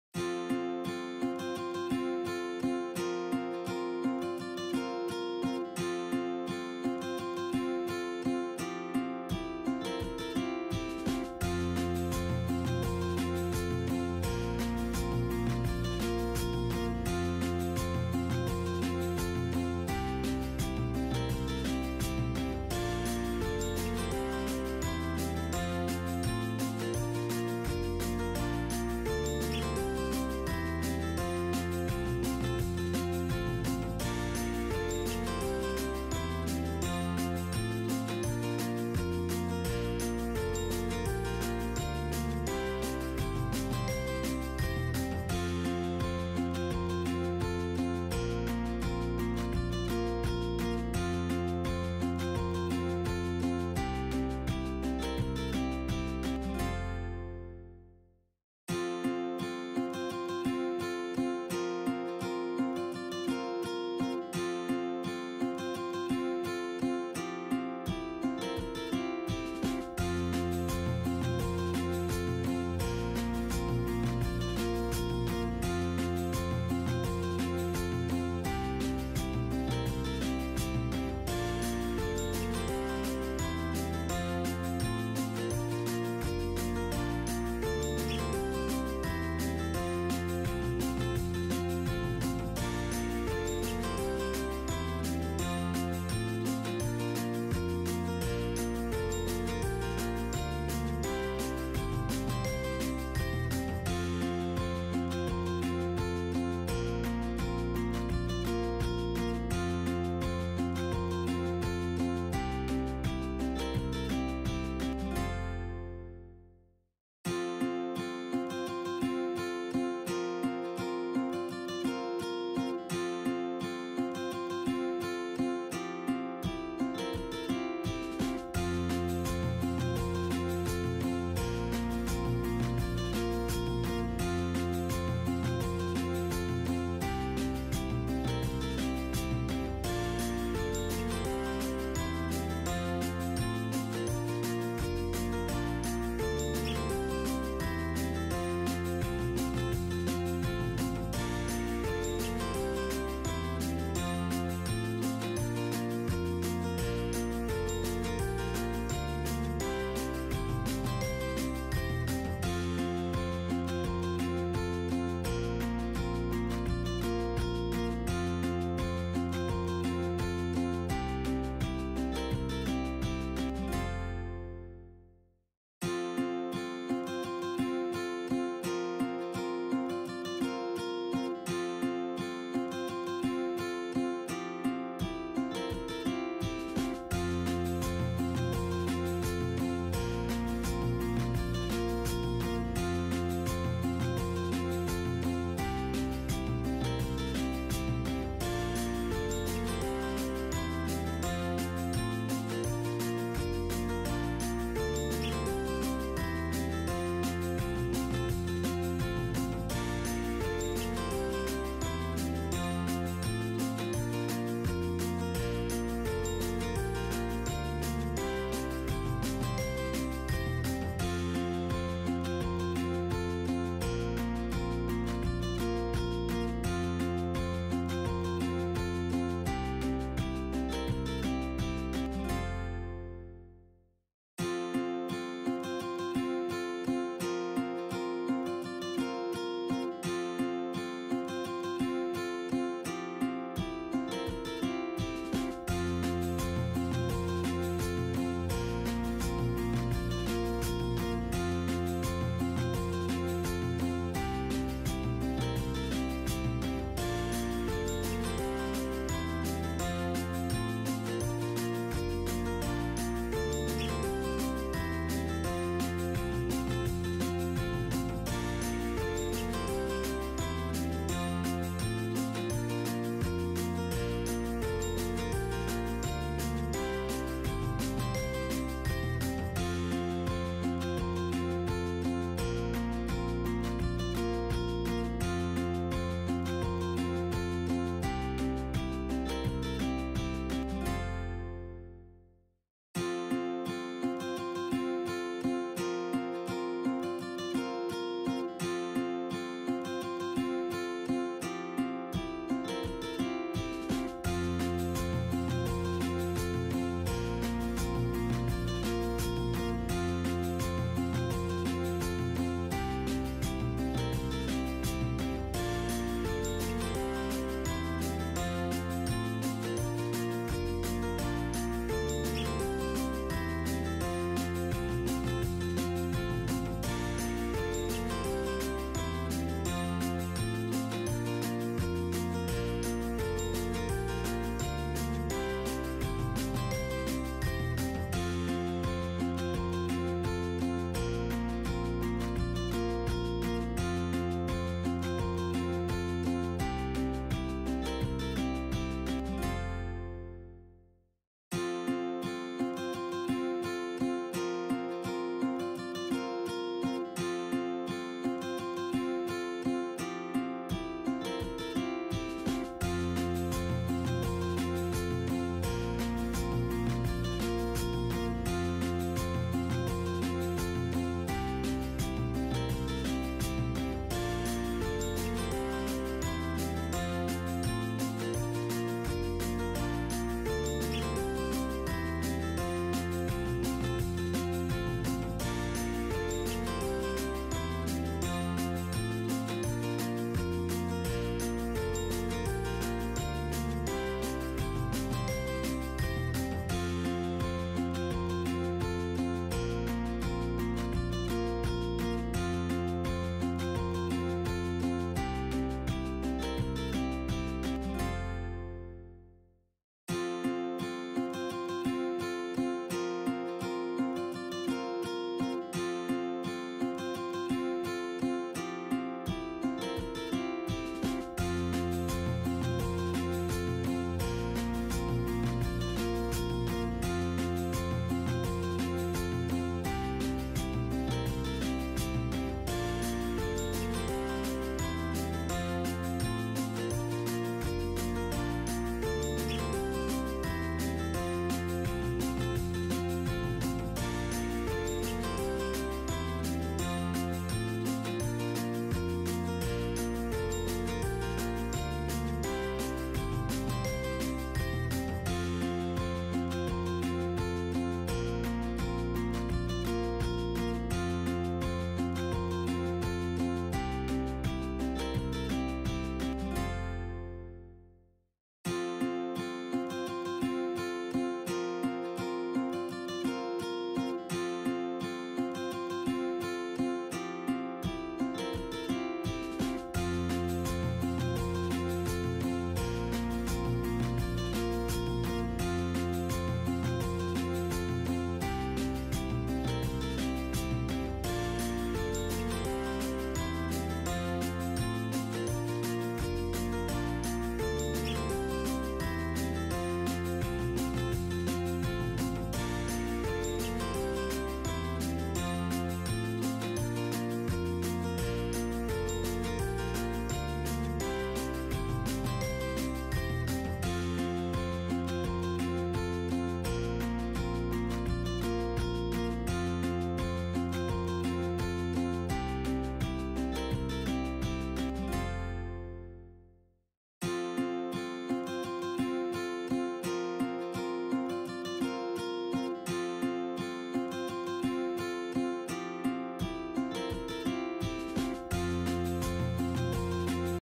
Dark Queen / Abstract Painting Demo / For Beginners / on Canvas / Acrylic Paints / Palette Knife
Demo of abstract painting for beginners “dark queen”  on canvas using acrylic paints and palette knife